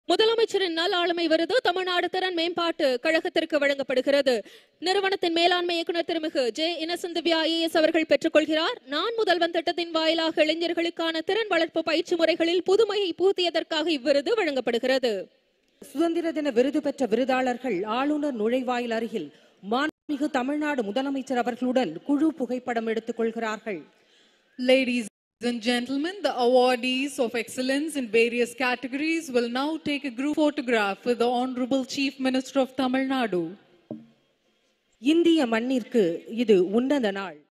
MD, TNSDC - Mrs. J Innocent Divya, IAS, has been honored with the "Good Governance Award" by the CM.
We are immensely proud to share that the Managing Director of the Tamil Nadu Skill Development Corporation, Mrs. J Innocent Divya, IAS, has been honored with the “Good Governance Award” by the Honourable Chief Minister of Tamil Nadu on this 78th Independence Day.

This award is a testament to our Managing Director’s visionary leadership and the unwavering commitment of our entire team in driving youth skill development across Tamil Nadu. Through the Naan Mudhalvan Skill initiative, we continue to empower the next generation, paving the way for a skilled and self-reliant India.